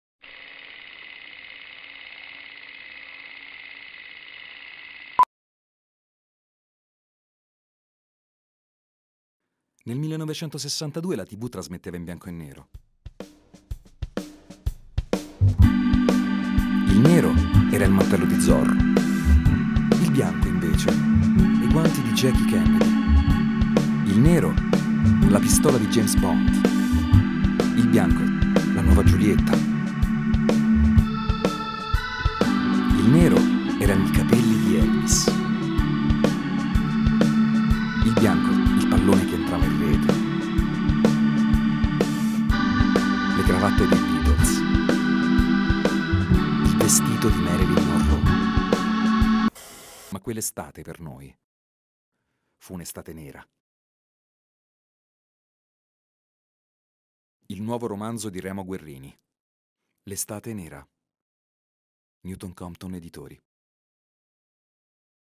L'estate nera - Remo Guerrini - il booktrailer
Da questo libro il film Eppideis

All'inizio sembrava solo un gioco

Un'estate come tante, un paesino come tanti
Un gruppo di ragazzini insolitamente crudele
Trent'anni dopo, nessuno di loro ha dimenticato quell'estate...

Massimino, Eva, Attila, Saturnina e poi Canavesio, Federico, Santino e Giusi sono ancora dei bambini durante quella torrida estate del '62.
Il giorno scherzano e scorrazzano per le strade di Altavilla, un paesino del Monferrato, e la sera dopo cena Carosello e a letto. Hanno solo dodici anni ma si sentono già grandi su quel muretto e perseguitare Beniamino il matto, per sentirlo imprecare e urlare, all'inizio è solo un gioco innocente e nessuno pensa davvero che finirà male durante quella maledetta domenica d'agosto, mentre imperversa un terribile temporale.
Passano trent'anni e il macabro ritrovamento dei resti di Beniamino nel cimitero di Altavilla rimette in moto i ricordi. E quei ragazzi del 1962, che la vita ha disperso e allontanato, sono costretti a ritrovarsi nei luoghi della propria infanzia. Diventando i protagonisti di una imprevista, improvvisa, orribile resa dei conti.

«L'estate nera è un capolavoro del giallo italiano. Geniale, appassionante, mozzafiato. Remo Guerrini cattura il lettore come solo i grandi scrittori sanno fare.»
L'editore

«Una rivelazione italiana che va oltre le etichette.»
Oreste del Buono

Da questo libro il film in uscita Eppideis
con Gianmarco tognazzi

Un'estate italiana. Molto più di un giallo
Un affresco nostalgico ma spietato del mitico 1962, quando un gruppo di ragazzini si affacciava di colpo alla vita adulta